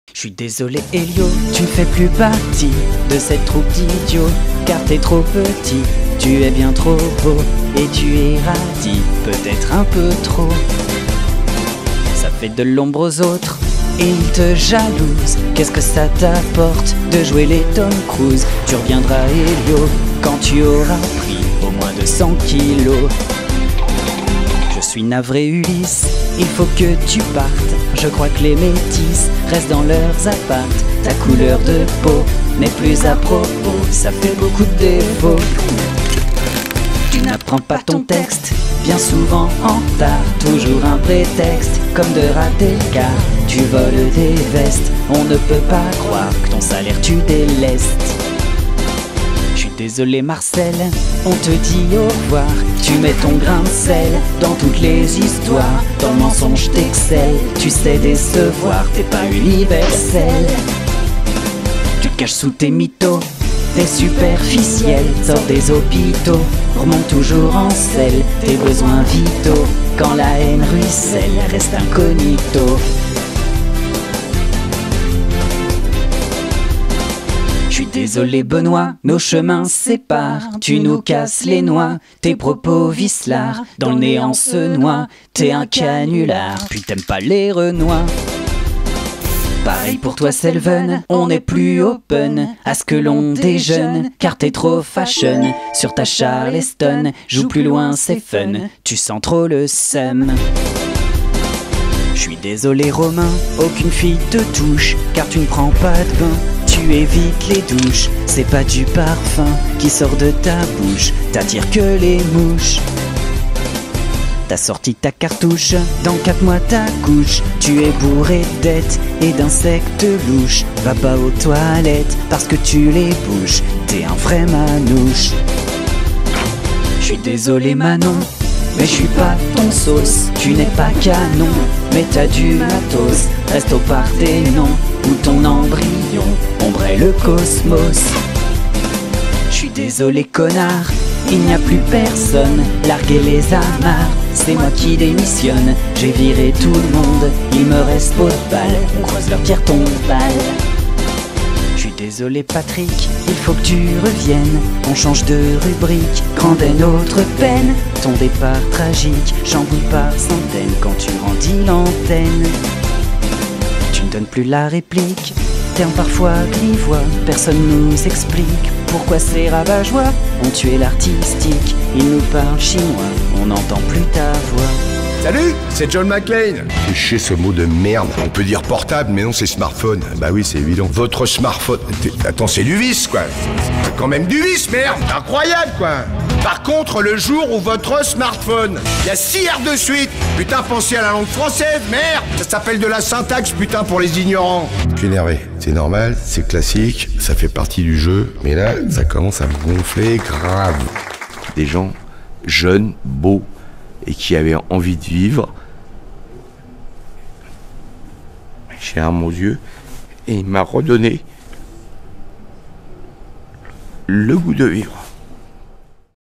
Licenciements Musicaux (Hommage à Patrick POIVEY)
Un brin d'humour et de nostalgie en évoquant pour rire quelques figures de proues emblématiques de la Troupe en Folie, avec un Hommage à Patrick Poivey qui les appréciait tous.
Avec le talentueux guitariste Hélio, mais également Benoît, Romain Jossant...

00:00 - 00:24 Hélio
00:24 - 00:49 Ulysse
00:49 - 01:17 Marcel
01:17 - 01:29 Benoît
01:29 - 1:42 Selven
01:42 - 2:07 Romain
02:07 - 02:19 Manon
02:19 - 02:32 Troupe
02:32 - 03:36 Patrick